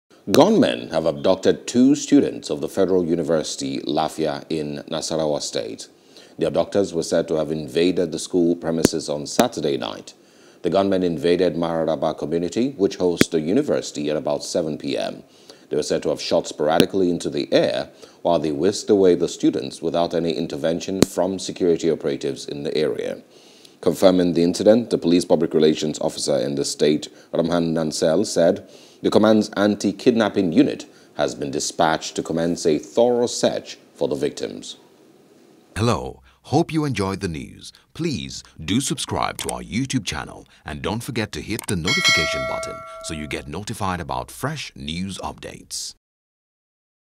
Nasarawa: Bandits Invade Federal Varsity, Kidnap Two Students, Demand N25m | NEWS
Gunmen have abducted two students of the Federal University, Lafia in Nasarawa State.